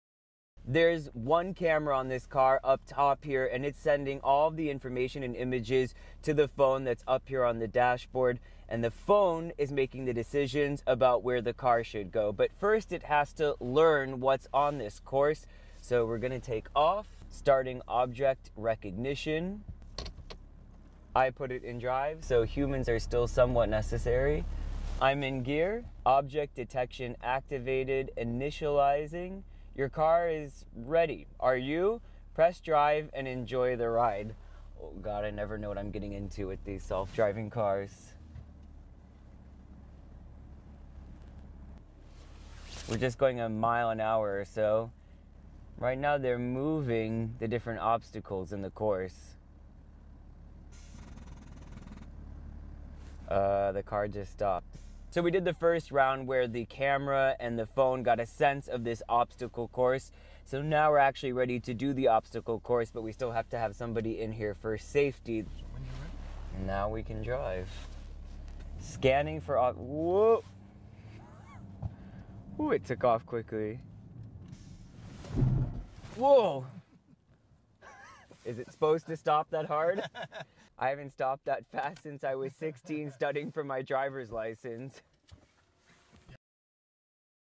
سيارة ذاتية القيادة يتحكم بها هاتف "هواوي"
لم يكن لدى مصنّع الهواتف "هواوي" طرازات جديدة لإظهارها في معرض "Mobile World Congress"، لكن الشركة استعرضت آخر ما توصلت إليه في قطاع التكنولوجيا من خلال تحكم هاتفها بسيارة "بورشه" من خلال قيادتها ذاتياً. ورغم أن التكنولوجيا التي تعمل عليها "هواوي" لم تصل بعد إلى المستوى المطلوب لإنجاز هذه المهمة، غير أن الفيديو يسلّط الضوء على أهمية الذكاء الاصطناعي وأثره في مساعدتنا في الحياة اليومية